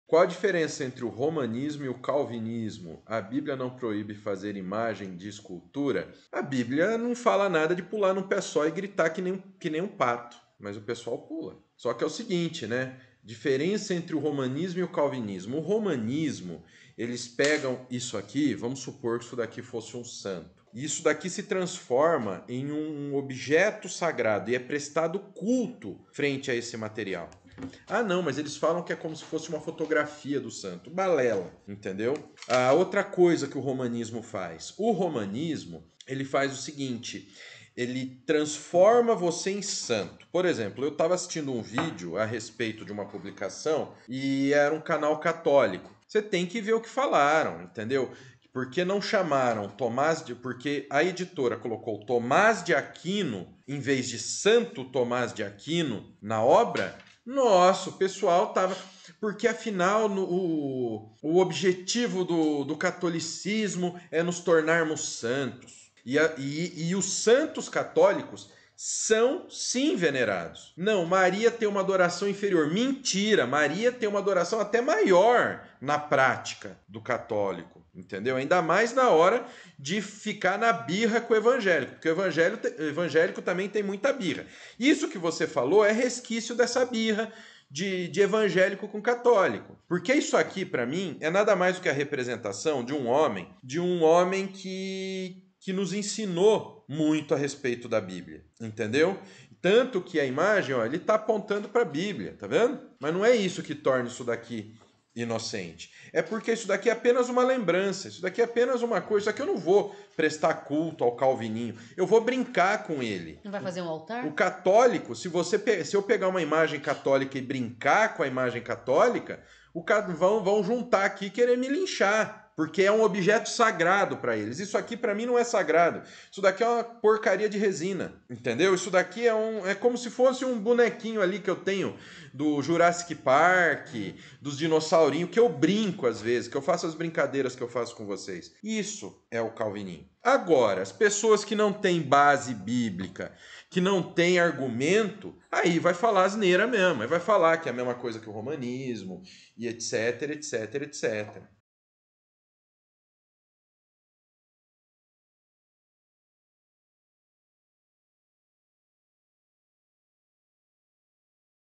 ROMANISMO X CALVINISMO
Neste vídeo retirando de nossa live de Perguntas e Respostas, responderemos: “Qual a diferença entre o Calvinismo e o Romanismo? A Bíblia não proíbe fazer imagens e esculturas?”

Confira!

Faça a Inscrição em nosso curso grátis "ESTUDO BÍBLICO EXAUSTIVO".